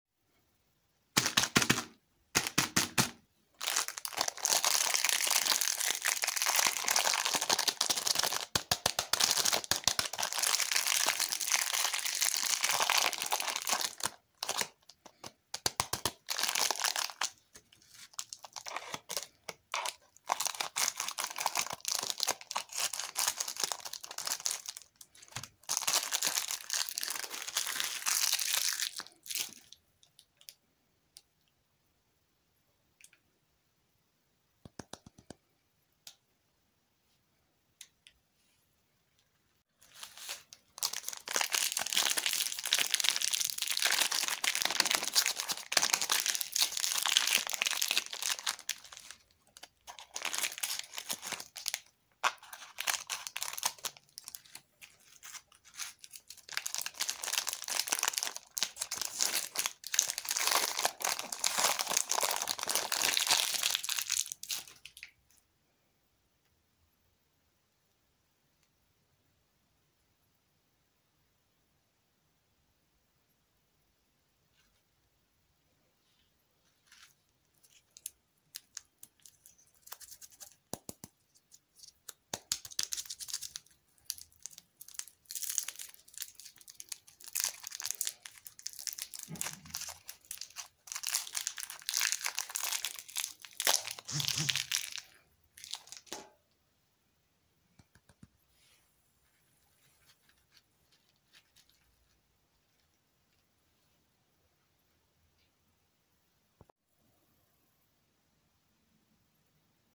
🍭 Learn Colors with lollipops and Sweets by Bibi Bu Bu, chupa chups, Snickers, Kinder Joy,
Hello friend this is my new video Learn Colors with lollipops and Sweets ASMR

🍭 Learn Colors with lollipops and Sweets by Cute Bibi, chupa chups, Snickers, Kinder Joy,